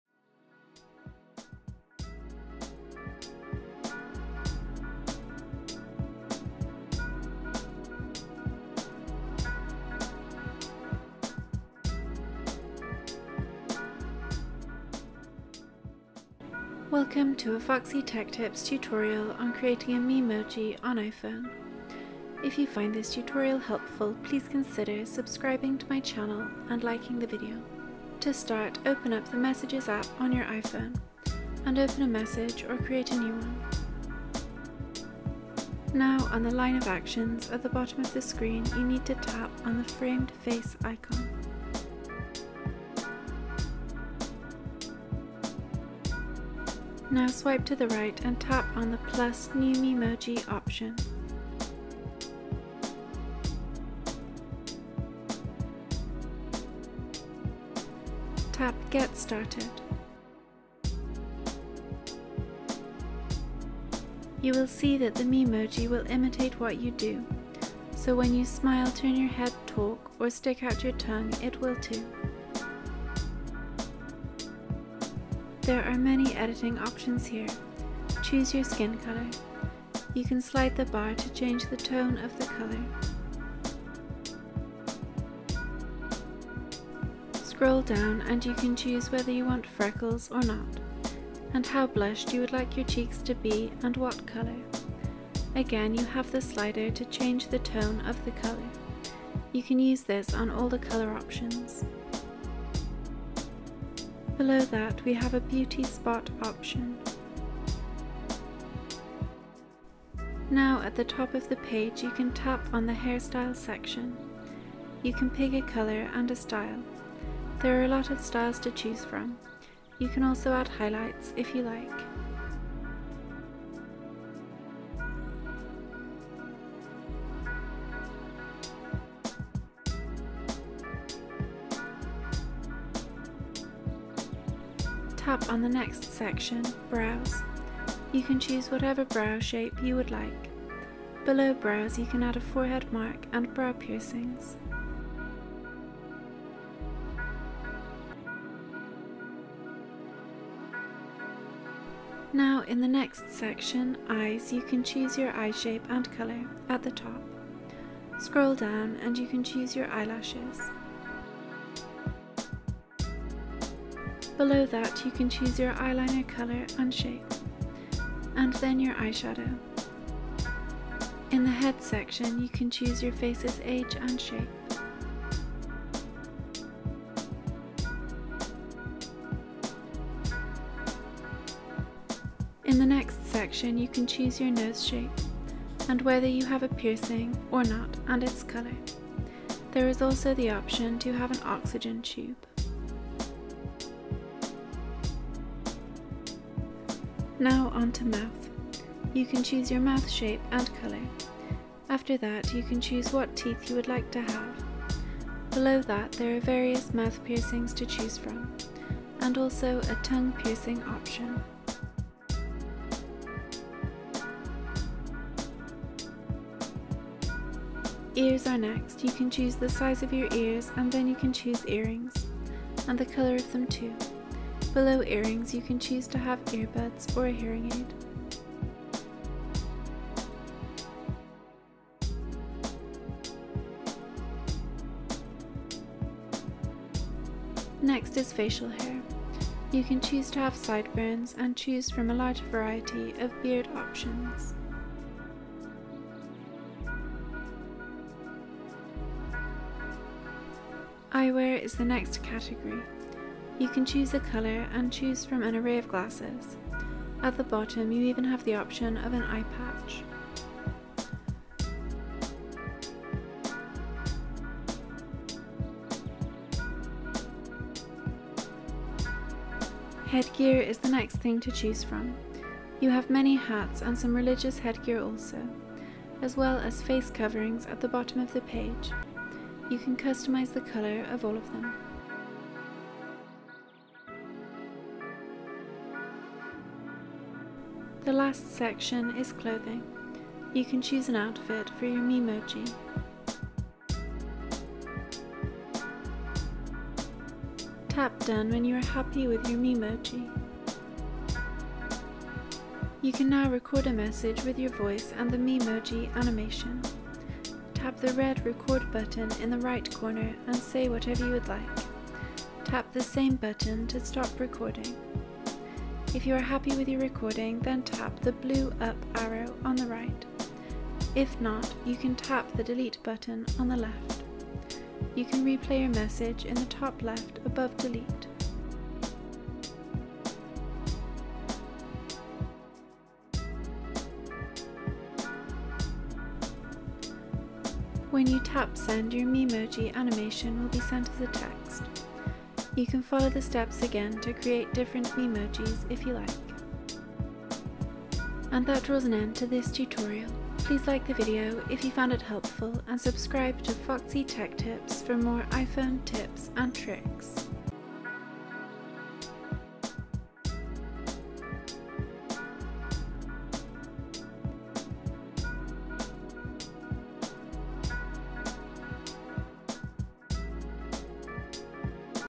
How To Create Memojis On iPhone
Easy to follow tutorial on creating new Memojis on iPhone (iOS). Want to create a new Memoji to use in chats on iPhone? Learn how to do it now!

Steps:

To start, open up the messages app on your iphone and open a message or create a new one.

Now on the line of actions at the bottom of the screen you need to tap on the framed face icon.

Now swipe to the right and tap on the plus ‘new memoji’ option.

Tap ‘get started’.

You will see that the memoji will imitate what you do, so when you smile, turn your head, talk or stick out your tongue it will too.

There are many editing options here, choose your skin colour, you can slide the bar to change the tone of the colour.

Scroll down and you can choose whether you want freckles or not and how blushed you would like your cheeks to be, and what colour. Again you have the slider to change the tone of the colour, you can use this on all the colour options.

Below that we have a beauty spot option.

Now at the top of the page you can tap on the hairstyle section. You can pick a colour and a style, there are a lot of styles to choose from! You can also add highlights if you like.

Tap on the next section, brows, you can choose whatever brow shape you would like. Below brows you can add a forehead mark and brow piercings.

Now in the next section, eyes, you can choose your eye shape and colour at the top. Scroll down and you can choose your eyelashes.

Below that you can choose your eyeliner colour and shape. And then your eyeshadow.

In the head section you can choose your face's age and shape.

In the next section you can choose your nose shape, and whether you have a piercing or not and its colour. There is also the option to have an oxygen tube.

Now onto ‘mouth’, you can choose your mouth shape and colour. After that you can choose what teeth you would like to have. Below that there are various mouth piercings to choose from, and also a tongue piercing option.

Ears are next, you can choose the size of your ears and then you can choose earrings, and the colour of them too. Below earrings you can choose to have earbuds or a hearing aid.

Next is  facial hair. You can choose to have sideburns and choose from a large variety of beard options.

Eyewear is the next category, you can choose a colour and choose from an array of glasses. At the bottom you even have the option of an eyepatch!

Headgear is the next thing to choose from, you have many hats and some religious headgear also, as well as face coverings at the bottom of the page. You can customize the color of all of them.

The last section is clothing, you can choose an outfit for your memoji.

Tap done when you are happy with your memoji.

You can now record a message with your voice and the memoji animation. Tap the red record button in the right corner and say whatever you would like. Tap the same button to stop recording. If you are happy with your recording then tap the blue up arrow on the right. If not you can tap the delete button on the left. You can replay your message in the top left, above delete.

When you tap send your memoji animation will be sent as a text. You can follow the steps again to create different memojis if you like.